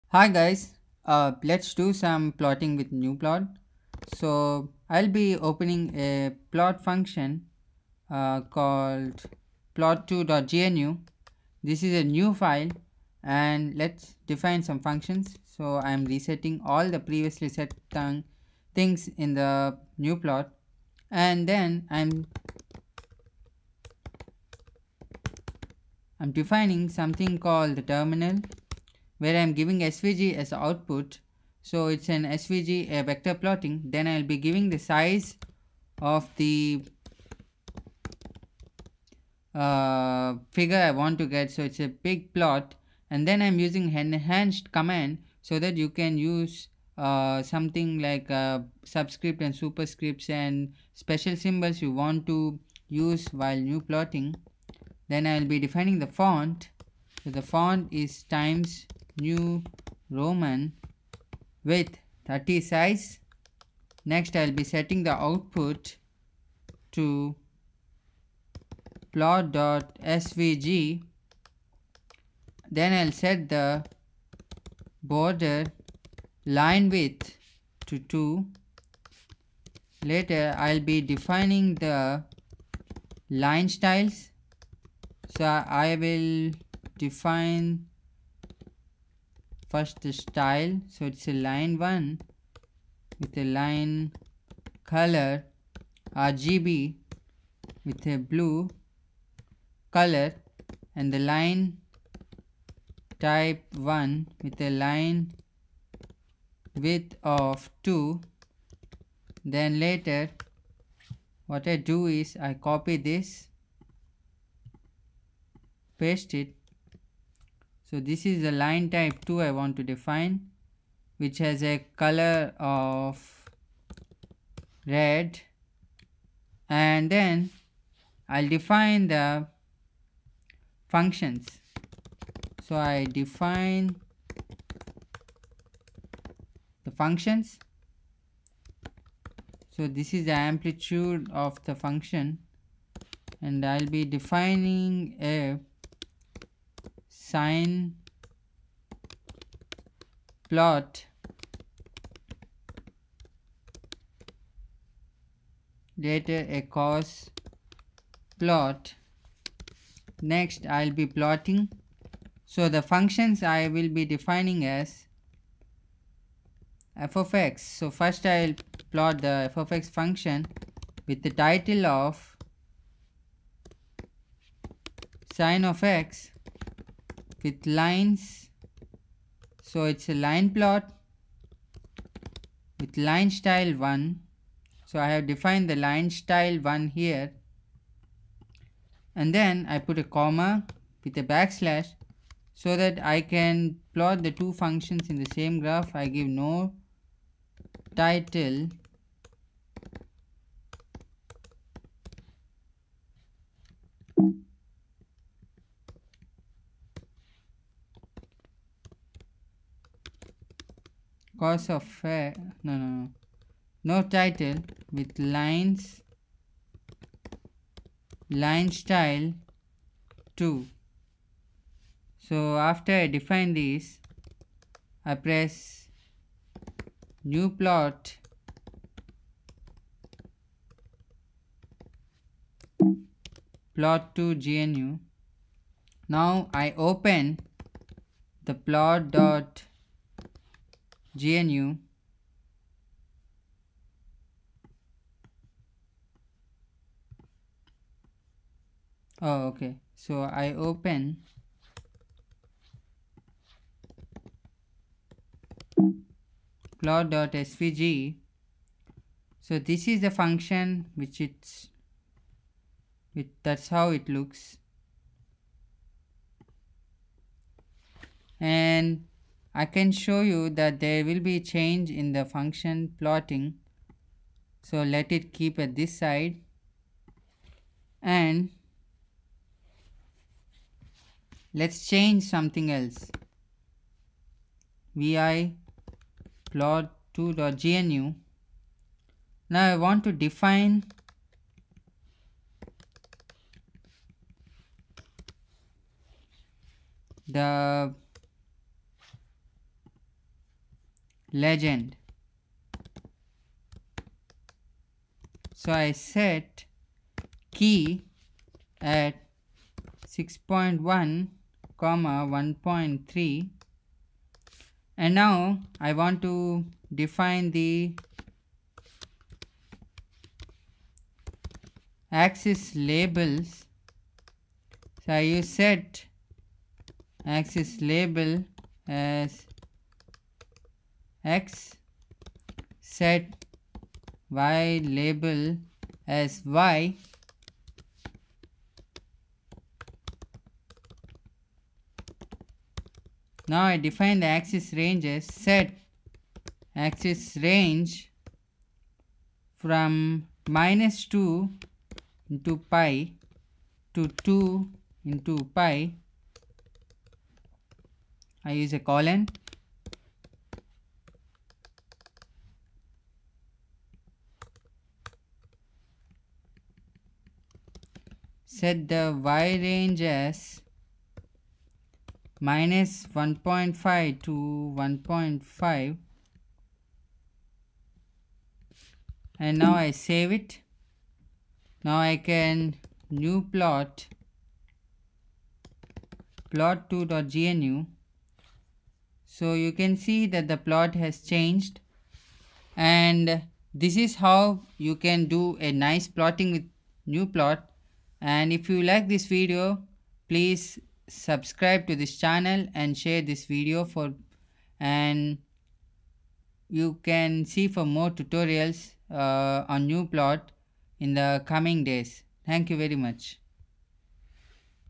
GNUPLOT | Basic Tutorial 01 | Plotting functions |Vector plotting | legends | axes labels
Tutorial 01 to use GNUPLOT for making vector plots

This video covers how to make basic plots with gnuplot, particularly this video covers on how to plot functions on gnuplot.

Enhanced options makes us able to use subscripts, superscripts, and special symbols like alpha, beta, gamma on GNUPLOT